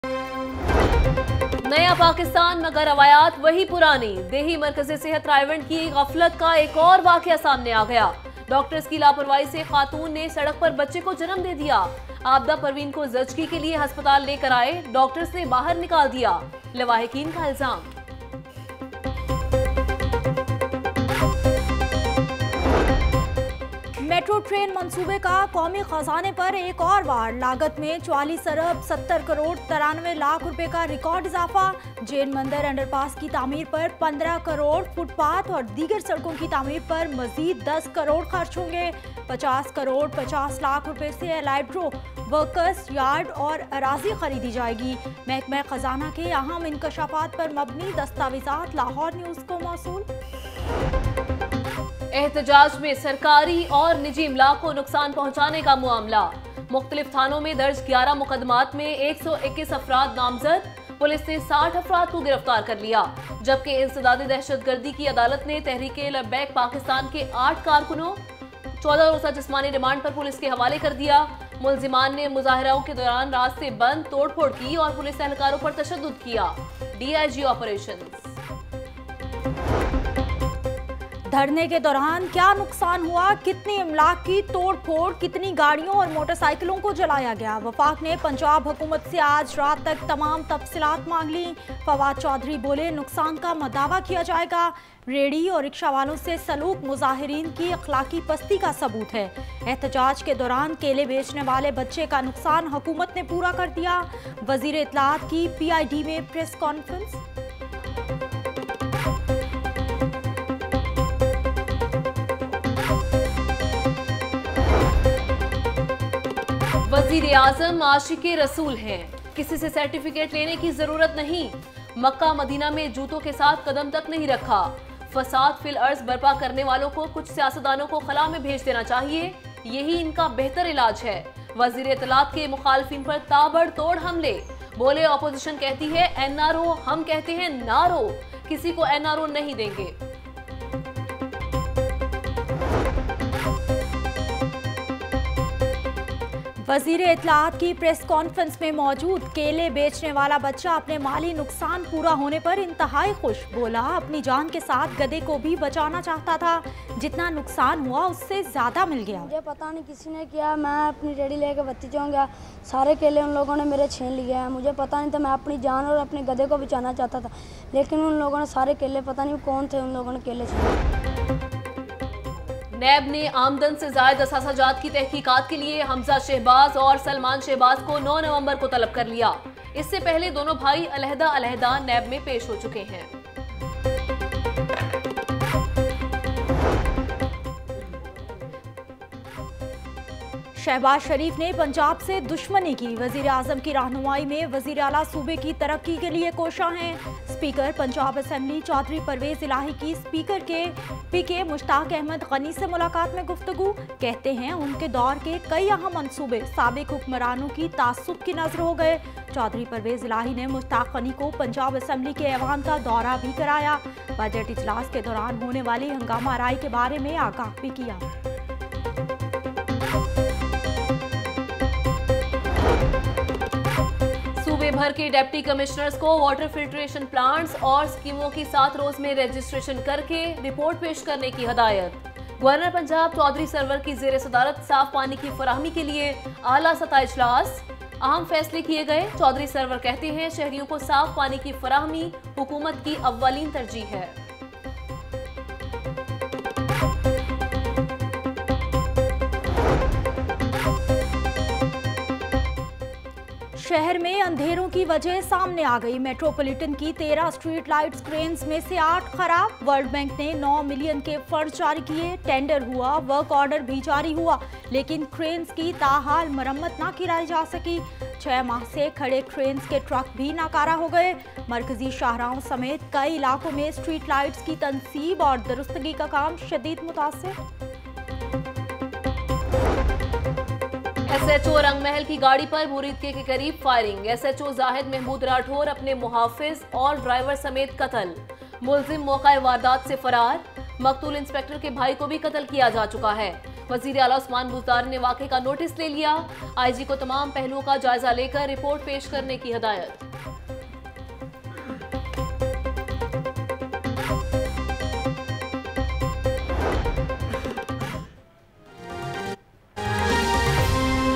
06 PM Headlines Lahore News HD – 5th November 2018
Watch Latest News:
Fawad Chaudhry launches action against violent protesters, Gas load shedding pstarts in different areas across city, Petrol prices witness sharp increase, Dolphin Force officer opens fire at local, Poultry prices witness sharp increase, PPP supports PML-N candidate in Senate Elections, NAB Court approves 10-days physical remand of Shehbaz Sharif, Dry Fruit prices increases, PM Imran Khan makes successful visit at China, Nawaz Sharif leaves from Jati Umra, Students block canal road, Immense traffic jam in Lahore, Scuffle breaks out between locals and traffic wardens, Dollar prices witness sharp increase, Oil tanker collides at Waris Meer underpass and much more.